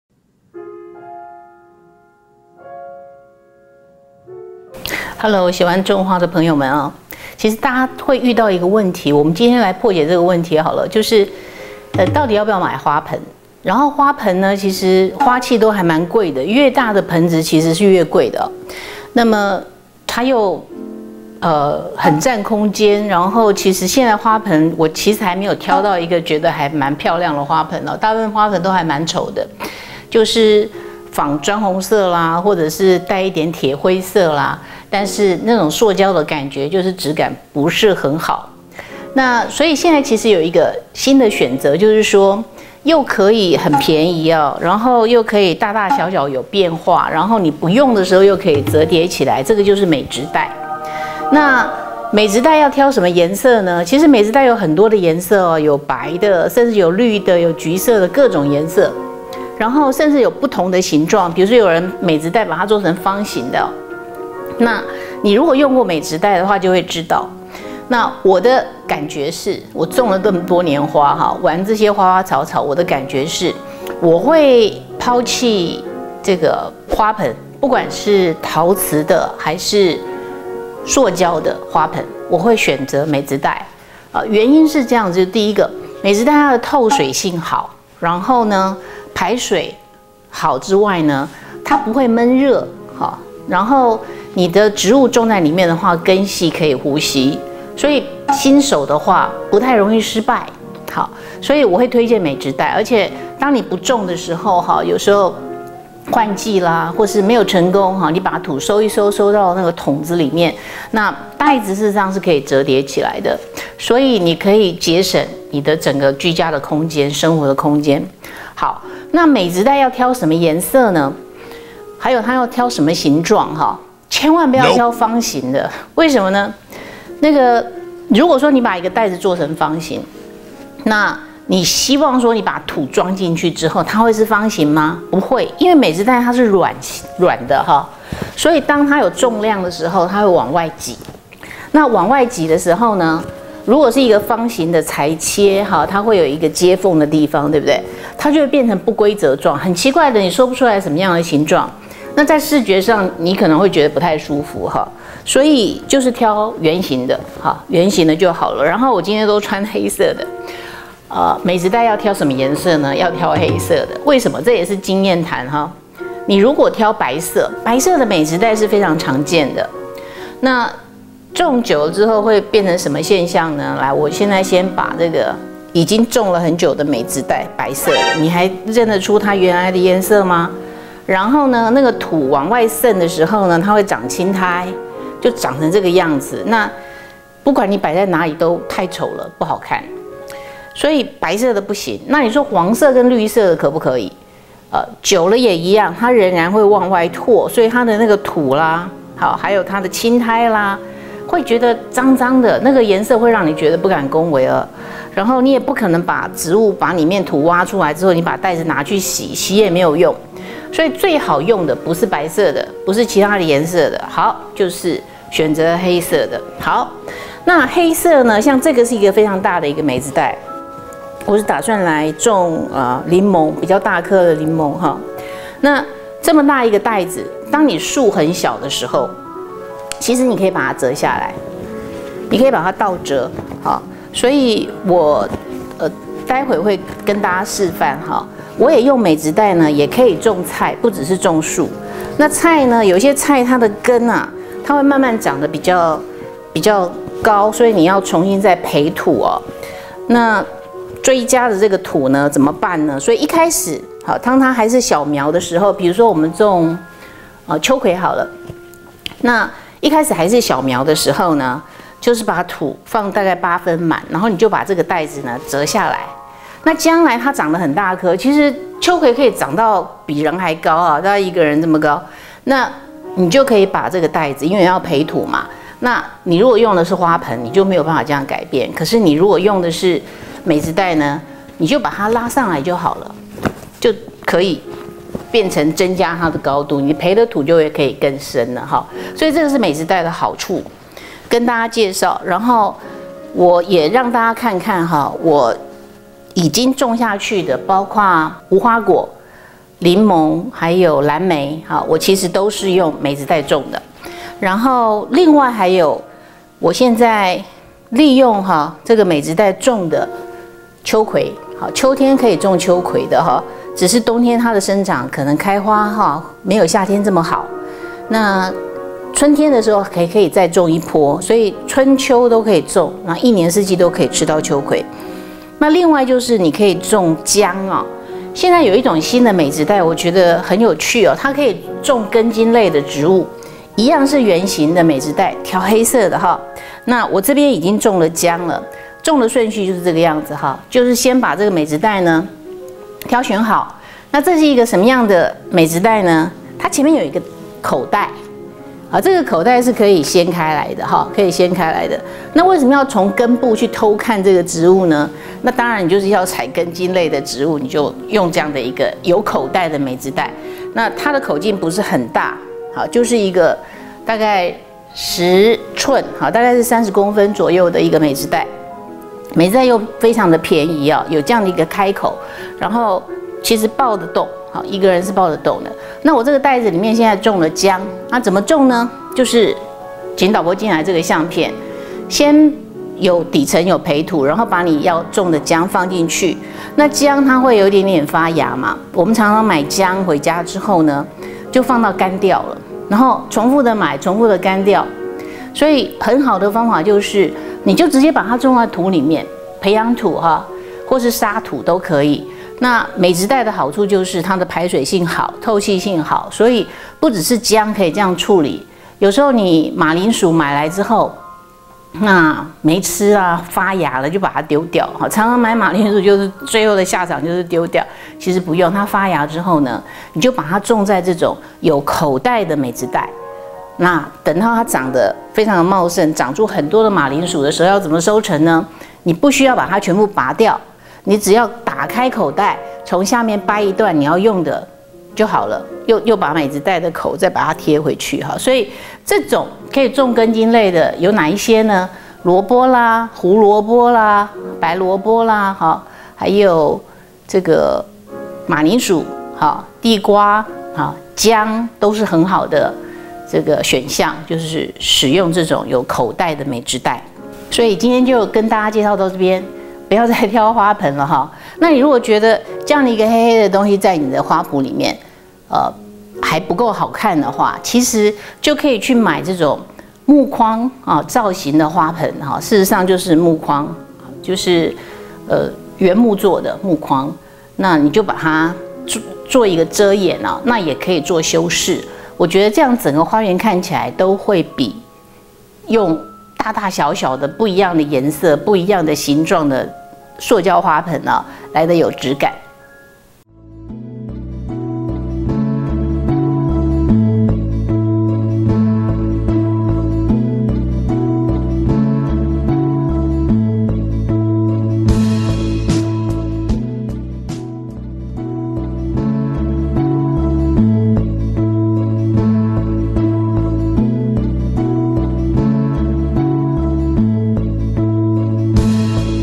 美觀又實用的美植袋｜童文薫的私廚
在家拈花惹草免不了需要器具來裝你辛苦栽種的大小植物，但是不喜歡笨重又不美觀的花盆怎麼辦?可以試試輕便好用、又容易收納的美植袋!